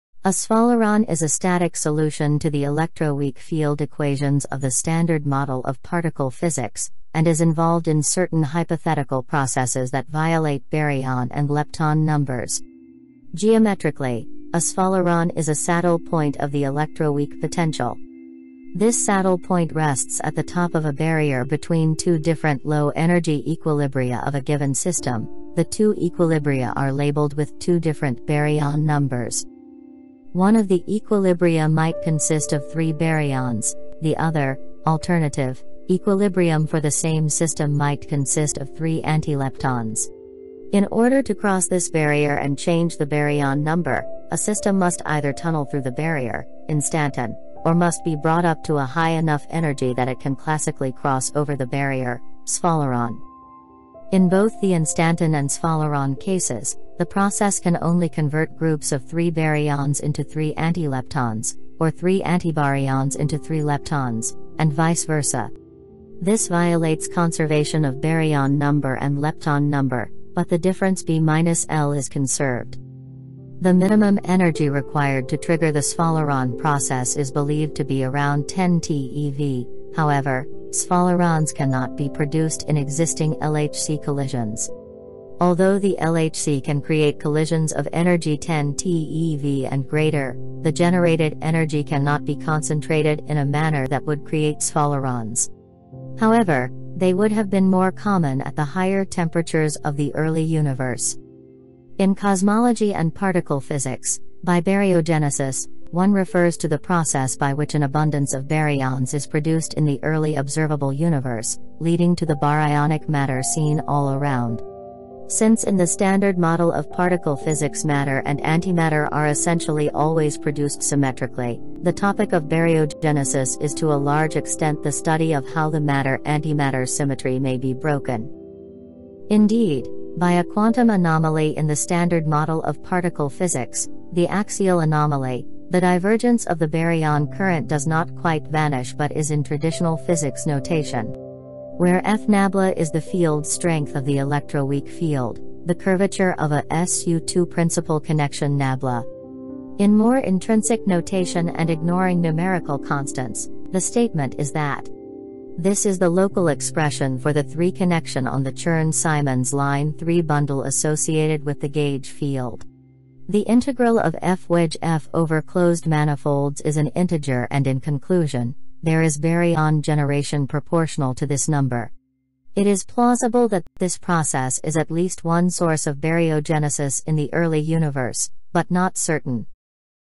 Sphaleron and electroweak baryogenesis | A quantum anomaly in the Standard Model of particle physics
A sphaleron is a static solution to the electroweak field equations of the Standard Model of particle physics, and is involved in certain hypothetical processes that violate baryon and lepton numbers. Geometrically, a sphaleron is a saddle point of the electroweak potential...

Video by LukasMediaProduction from Pixabay_Slomo

0:00 What is sphaleron?
0:54 Baryon and lepton number violation
1:41 Baryogenesis